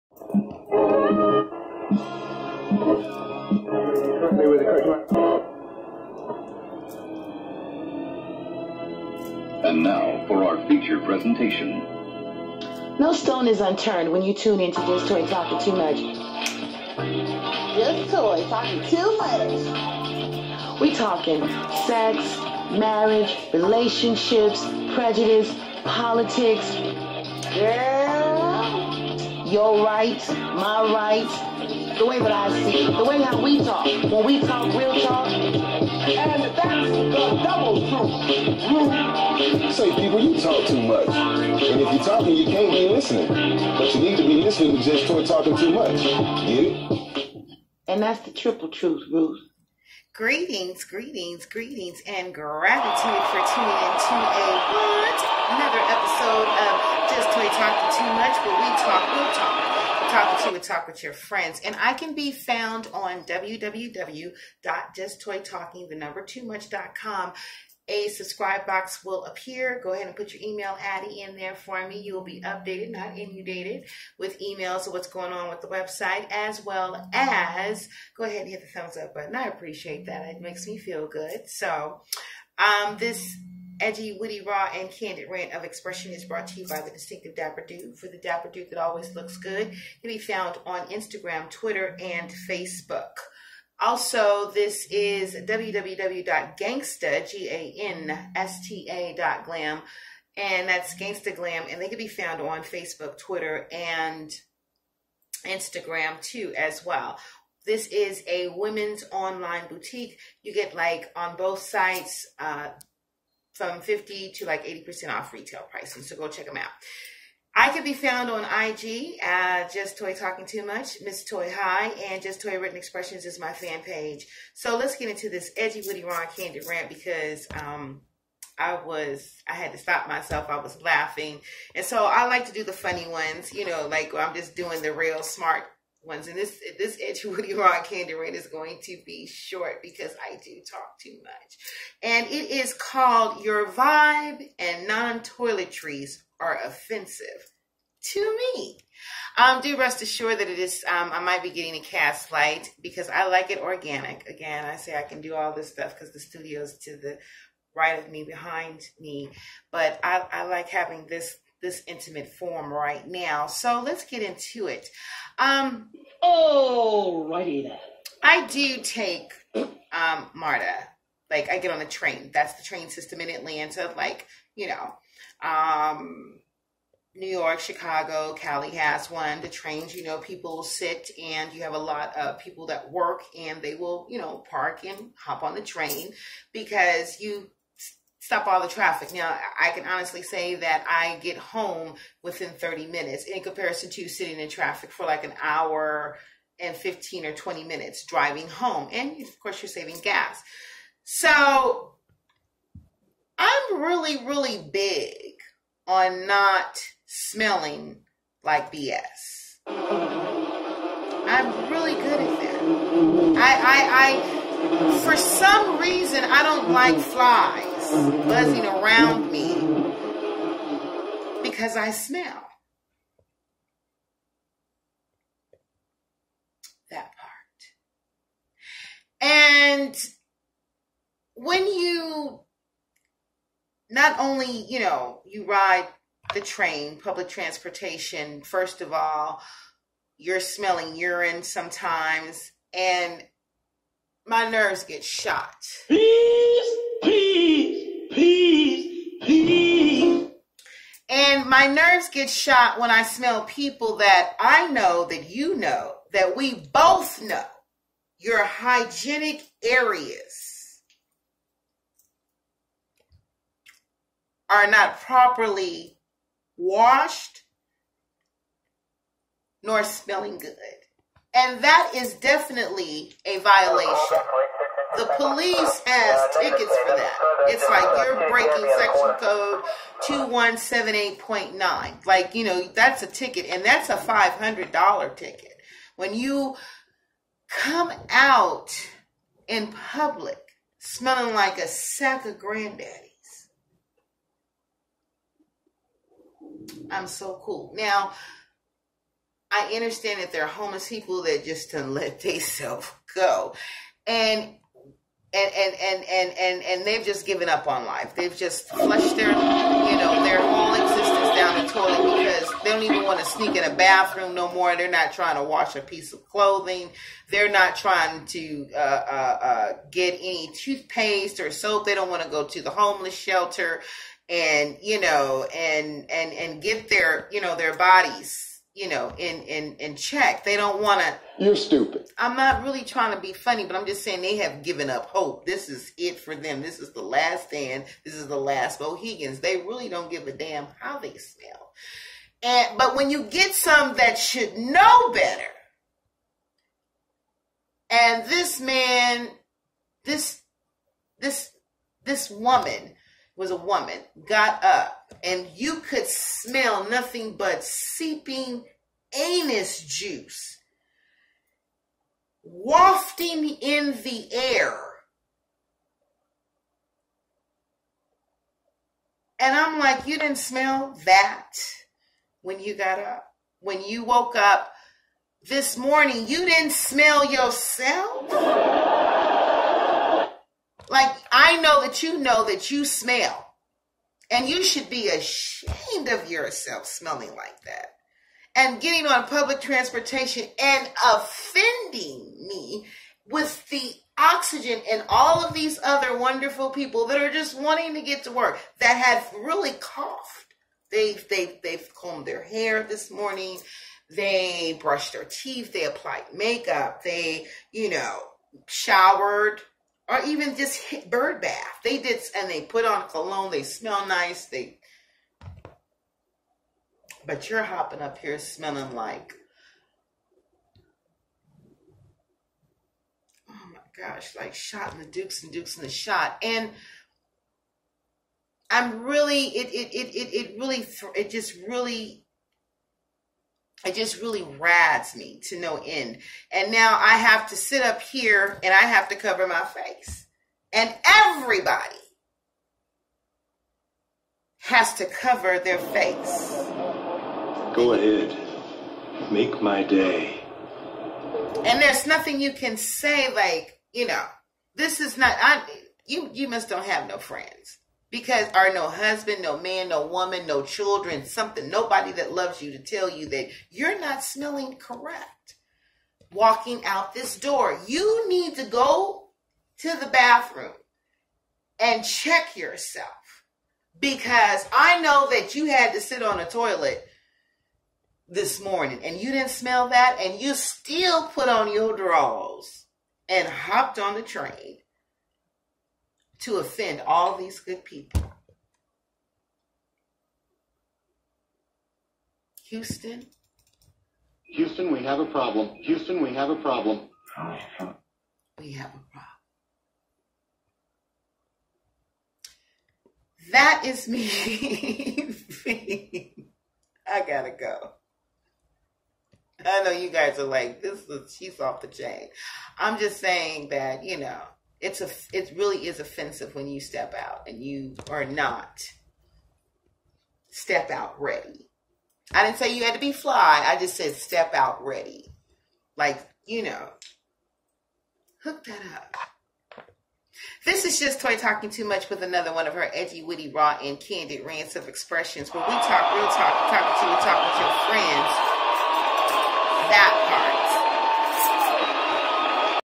YOU'RE VIBE AND NON-TOILETRIES ARE OFFENSIVE!!!!
Let us please be cognitive of other's when we are in public! This, of course, does not include the homeless and downtrodden.  This is for those that know they are not bathing, smelling like roses and unclean body odor. You know your body is not in check!